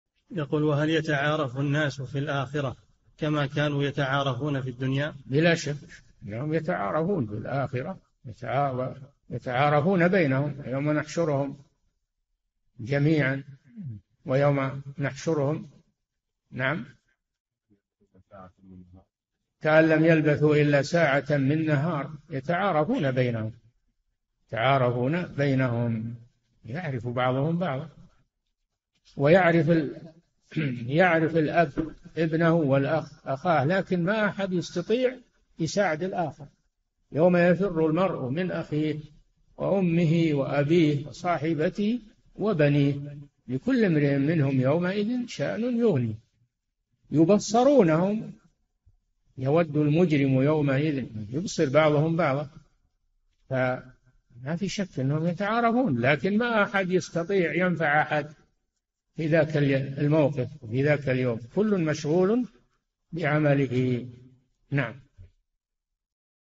[3242- 4717] هل يتعارف الناس في الآخرة كما كانوا يتعارفون في الدنيا ؟ - الشيخ صالح الفوزان
"[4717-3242] يَقُولُ: وهل يتعارف الناس في الآخرة كما كانوا يتعارفون في الدنيا؟
الجَوَابُ: بلا شك أنهم يتعارفون في الآخرة، ﴿وَيَوْمَ يَحْشُرُهُمْ كَأَنْ لَمْ يَلْبَثُوا إِلَّا سَاعَةً مِنَ النَّهَارِ يَتَعَارَفُونَ بَيْنَهُمْ﴾ [يونس : 45] فيعرف بعضهم بعضًا، ويعرف الأب ابنه والأخ أخاه لكن ما أحد يستطيع يساعد الآخر ﴿يَوْمَ يَفِرُّ الْمَرْءُ مِنْ أَخِيهِ (34) وَأُمِّهِ وَأَبِيهِ (35) وَصَاحِبَتِهِ وَبَنِيهِ (36) لِكُلِّ امْرِئٍ مِنْهُمْ يَوْمَئِذٍ شَأْنٌ يُغْنِيهِ﴾ [عبس: 34 - 37] ﴿يُبَصَّرُونَهُمْ يَوَدُّ الْمُجْرِمُ لَوْ يَفْتَدِي مِنْ عَذَابِ يَوْمِئِذٍ﴾ [المعارج: 11] يبصر بعضهم بعضًا فما في شك أنهم يتعارفون لكن ما أحد يستطيع ينفع أحدًا في ذاك الموقف وفي ذاك اليوم فكُلّ مشغول بعمله.

للاشتراك في قناة الشيخ صالح الفوزان
فتاوى المجالس في تفسير المفصل

للاستفسار والإقتراحات والدعم
رئيس الفريق
لمشروع كبار العلماء